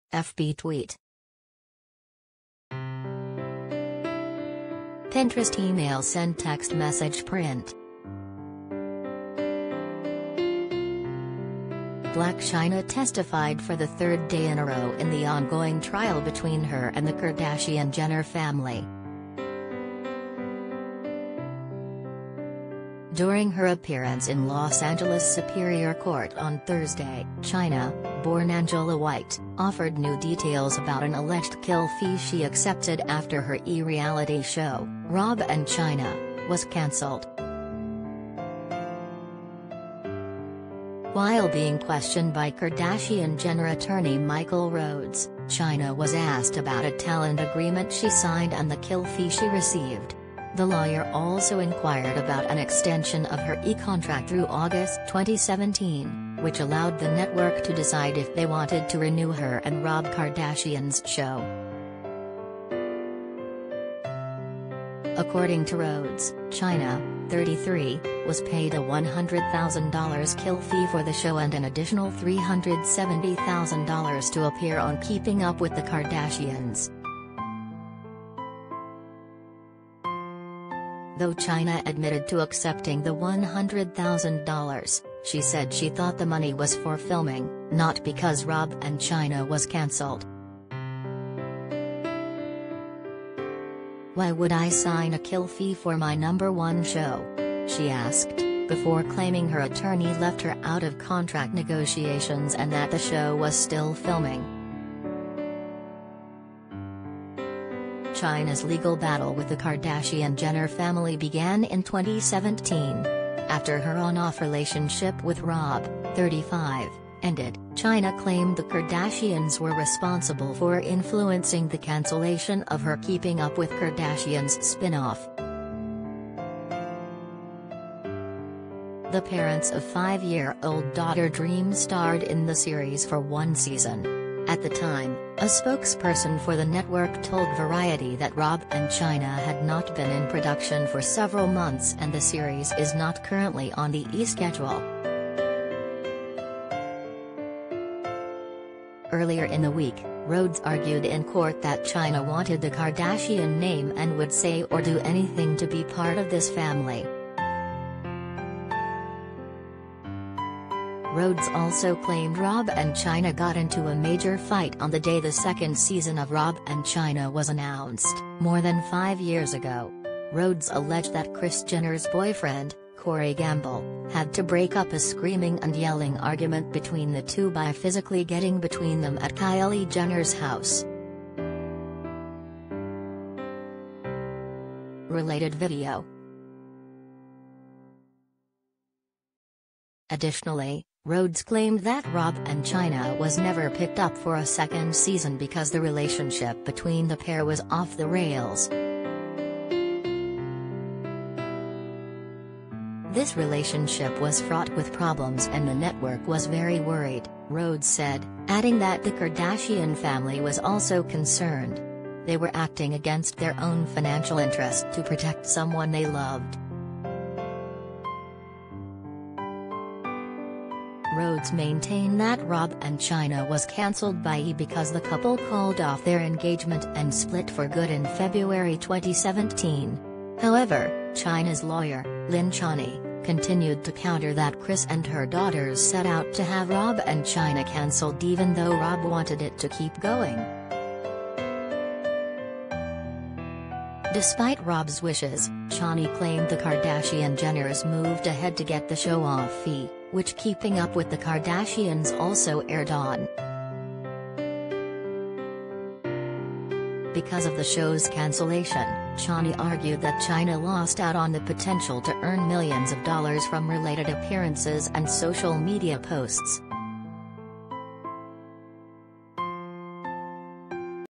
Blac Chyna Denies She Willingly Accepted $100,000 'Kill Fee' After Rob & Chyna Was Canceled
FB Tweet
Blac Chyna testified for the third day in a row in the ongoing trial between her and the Kardashian-Jenner family.
During her appearance in Los Angeles Superior Court on Thursday, Chyna — born Angela White — offered new details about an alleged "kill fee" she accepted after her E! reality show, Rob & Chyna, was canceled.
While being questioned by Kardashian-Jenner attorney Michael Rhodes, Chyna was asked about a talent agreement she signed